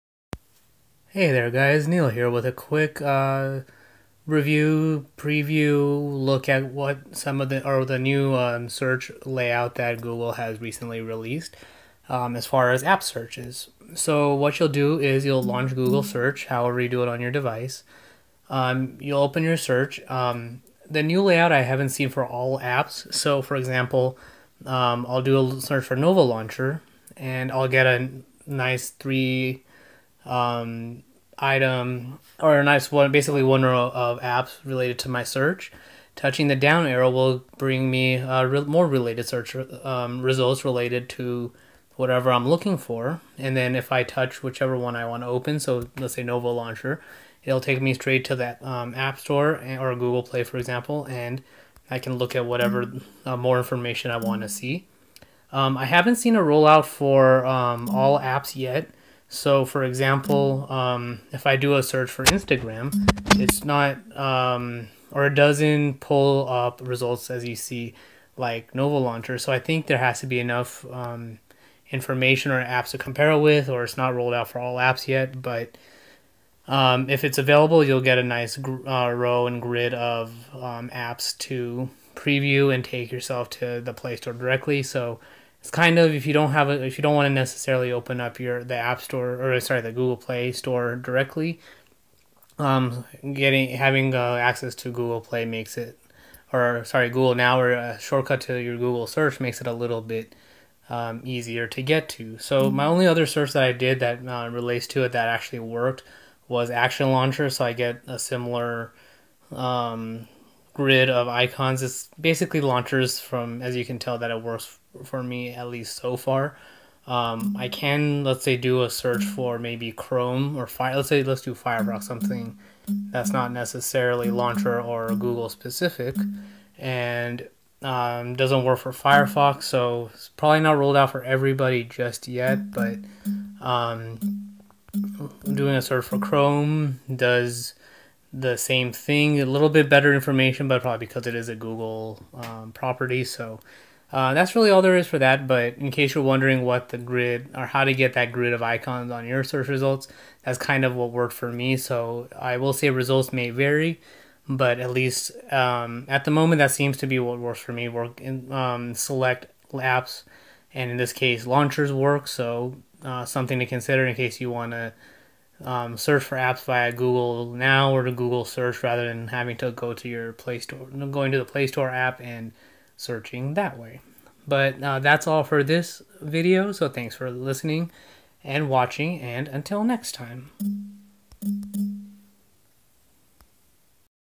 New app results UI on mobile
Here is a quick walkthrough of what the new grid UI looks like for app results on mobile. I used the most recent version of Google Now on Android 4.4.4. All I got the results on were Action Launcher and Nova Launcher so it may be a slow rollout or limited for now. I figure I'll use the disclaimer of results may vary as I tend to do more searches for custom launchers in Google Play than anything else I think.